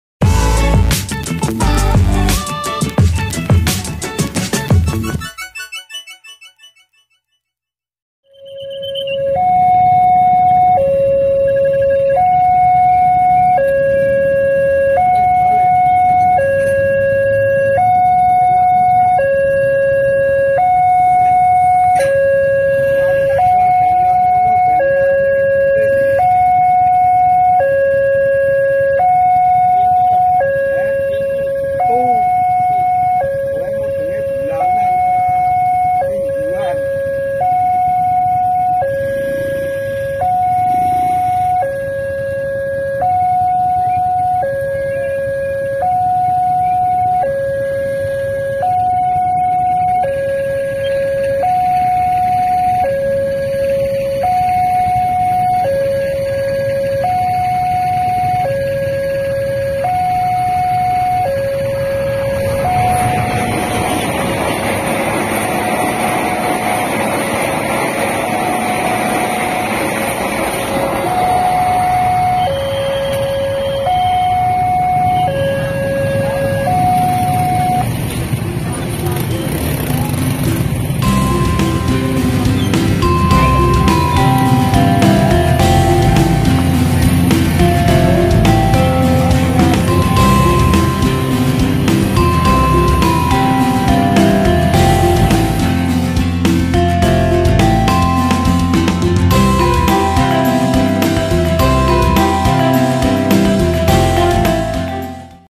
Ada Kereta Ngebut di Perlintasan Mayang Jawa Tengah
Kereta Api yang satu ini ngebutnya luar biasa. Semoga selalu aman, lancar, dan terkendali selamat sampai tujuan.